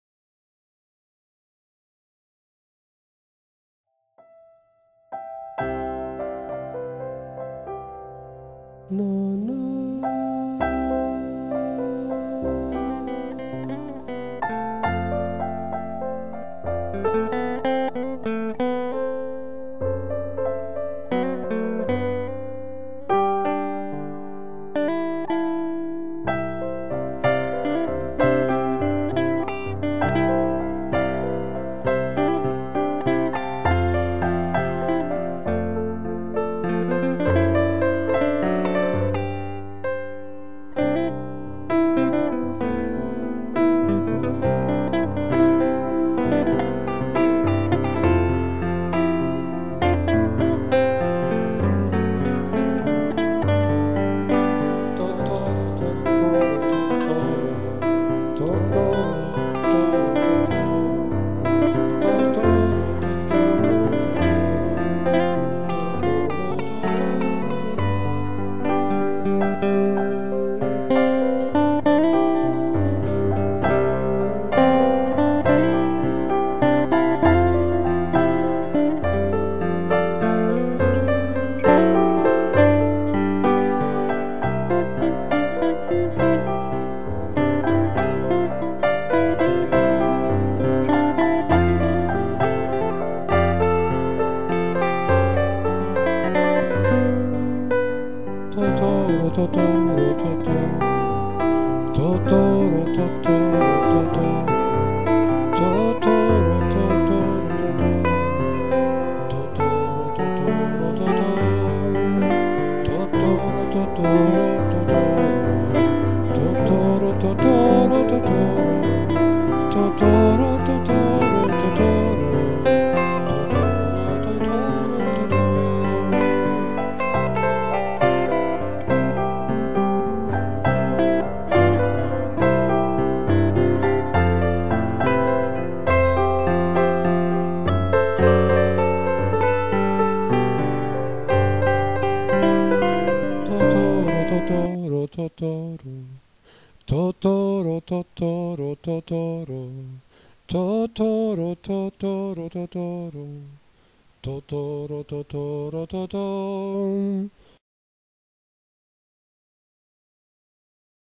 naughty ed - totoro inspired music.wmv
music inspired by Miyazakis Totoro. filming with my left hand and drawing with the right. the result is the slimmest Totoro ever